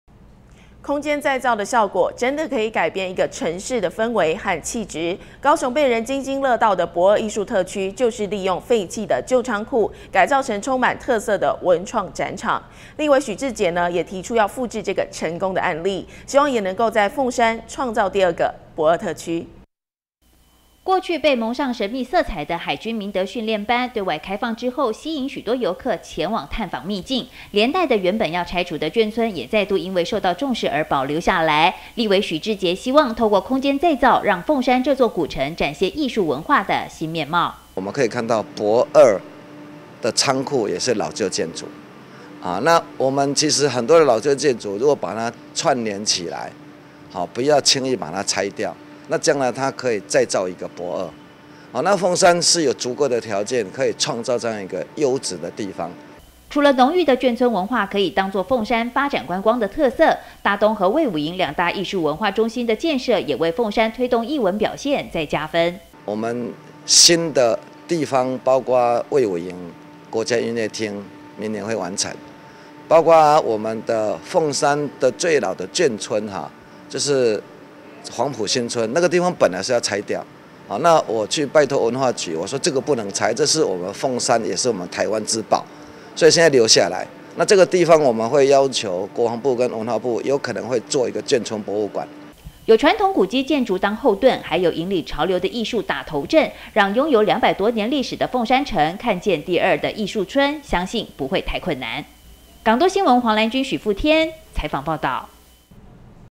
1030703【複製駁二經驗在鳳山 立委絕對有條件】TKTV高雄都會台港都新聞
空間再造的效果真的可以改變一個城市的氛圍和氣質,高雄被人津津樂道的駁二藝術特區,就是利用廢棄的舊倉庫,改造成充滿特色的文創展場,立委許智傑也提出要複製這個成功的案例,希望也能在鳳山創造第二個駁二特區.